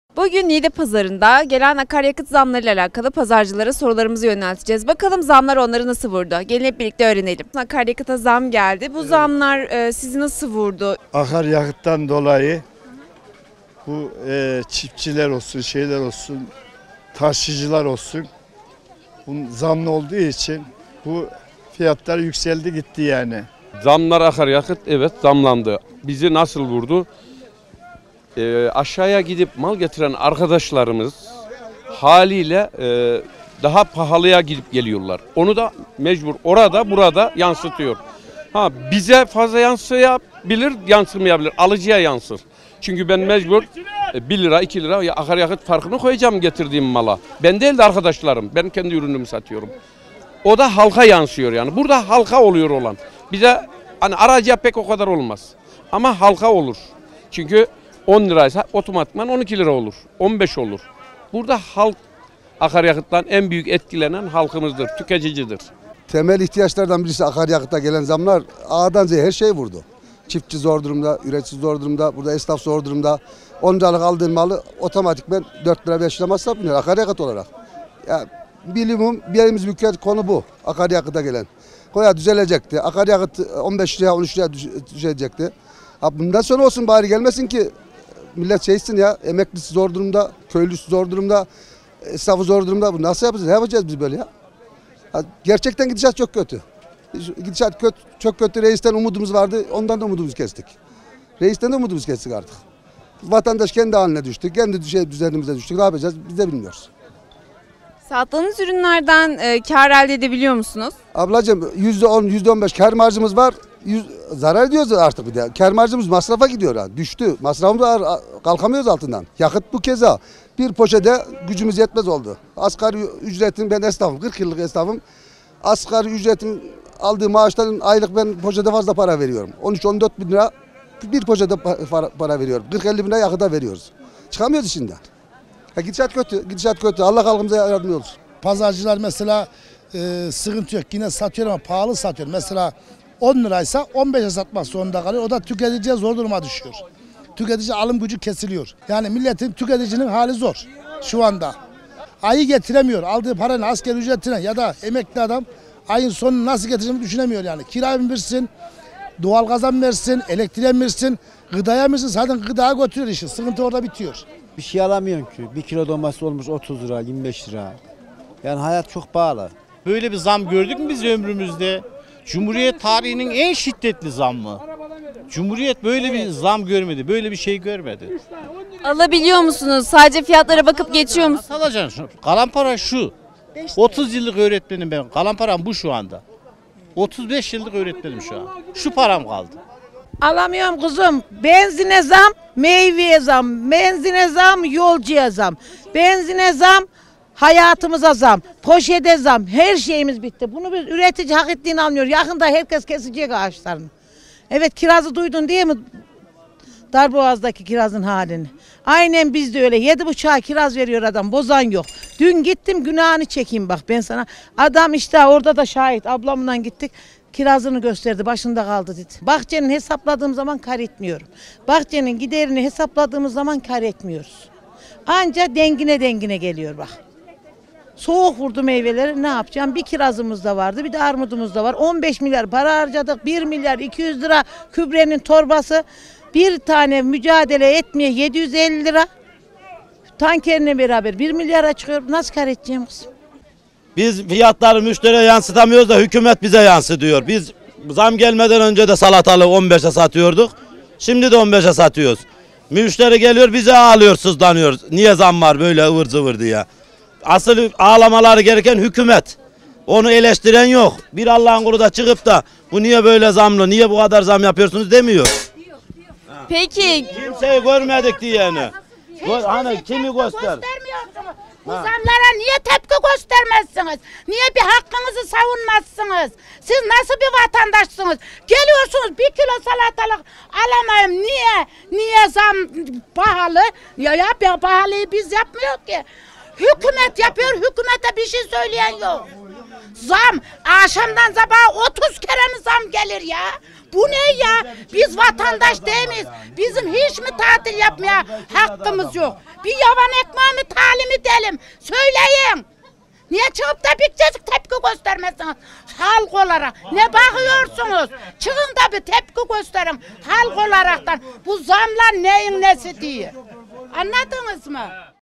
Vatandaşın Pazarda Zam Tepkisi
Niğde pazarcılarına ve vatandaşlara akaryakıt zamlarından nasıl etkilendiklerini sorduk.